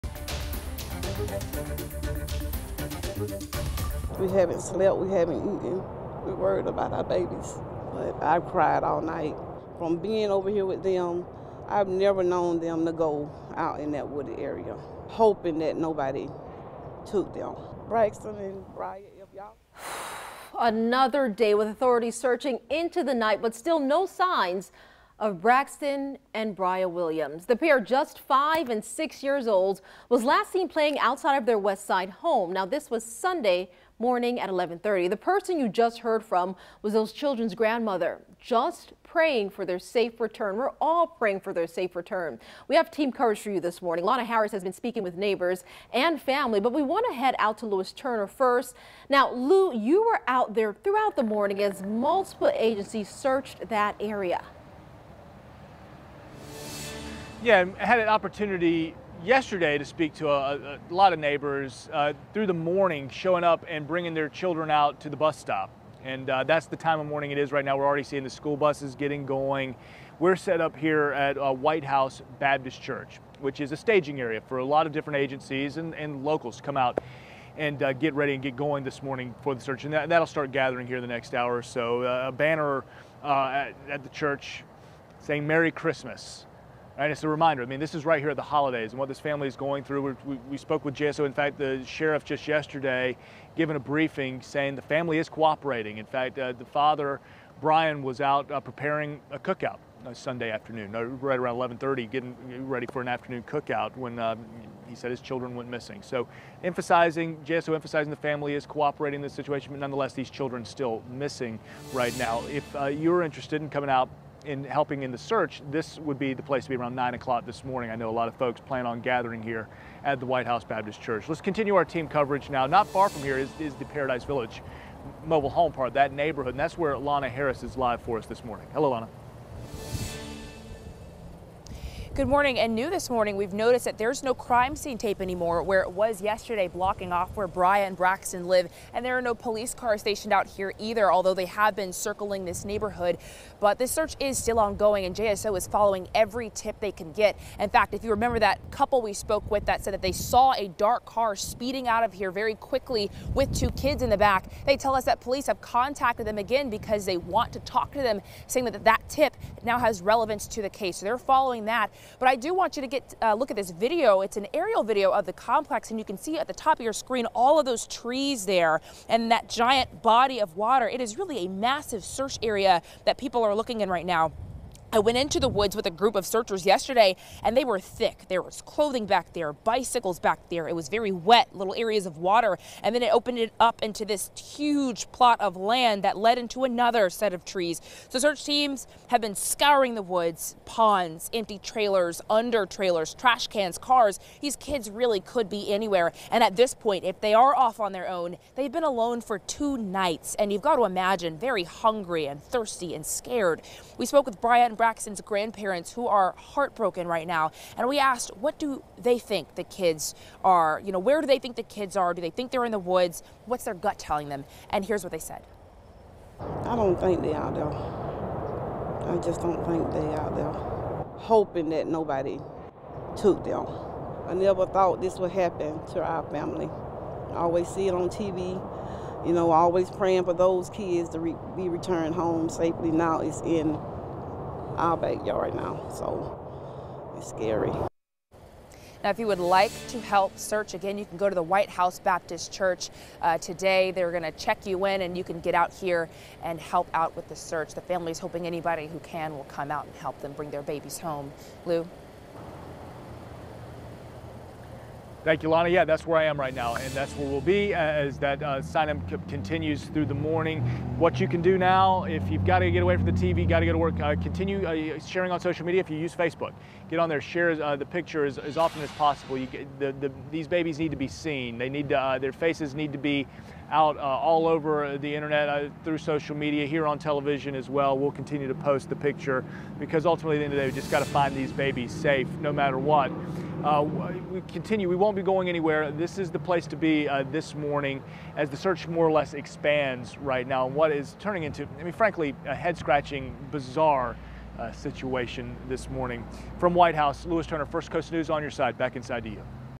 Day 3 in search for missing children from Westside
Still no sign of 6-year-old Braxton Williams and his little sister 5-year-old Bri'ya Williams. Here's what you need to know regarding the search for them.